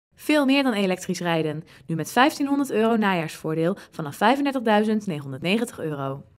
De Mitsubishi Outlander PHEV bij Kardol Autobedrijf
De Mitsubishi Outlander PHEV: veel meer dan elektrisch rijden! Nu met €1.500 najaarsvoordeel, vanaf €35.990. Kijk op